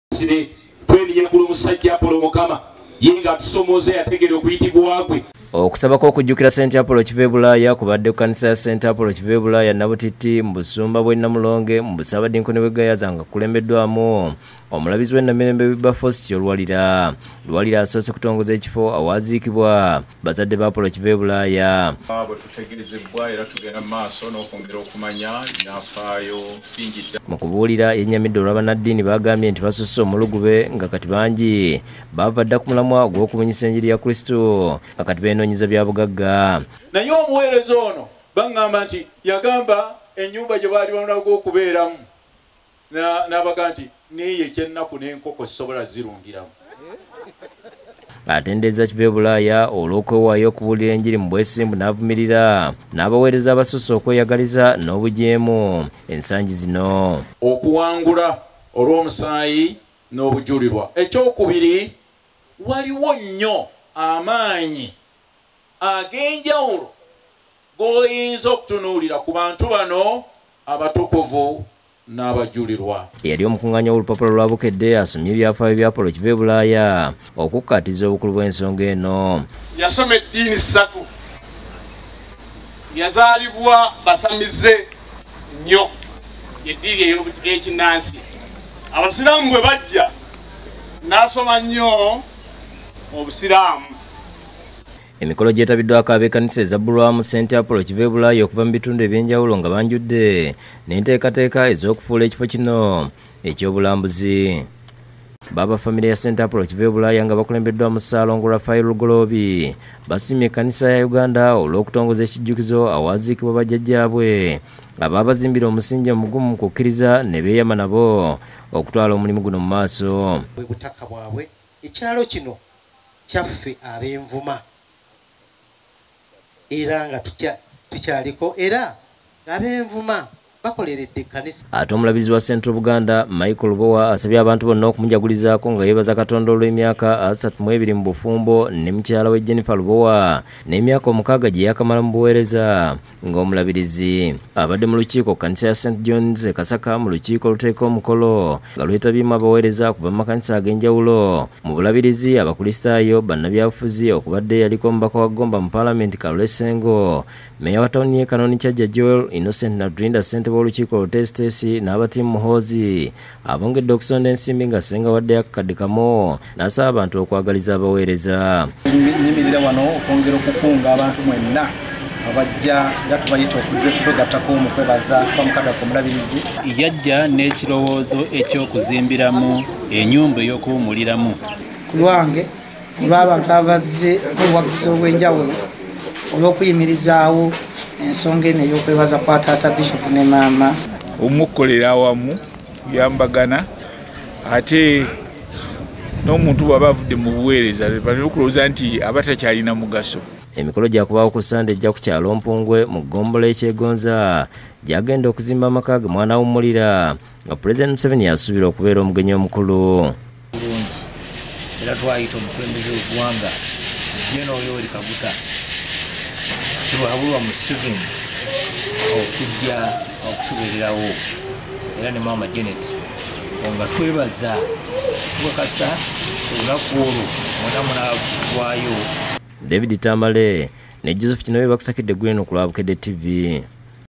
Agataliikonfuufu:Omulabirizi Kityo Luwalira Atabukidde abalina Omululu agamba bangi baava ku Mulamwa
Omulabirizi w'e Namirembe Wilberforce Kityo Luwalira atabukidde bannaddiini b'agambye nti basusse omulugube nga bangi baava dda ku mulamwa gw'okubunyisa enjiri ya Kristu beenoonyeza byabwe. Okwogera bino abadde mu kusaba okw’okujjukira St. Apollo Kivebulaya e Nabutitti.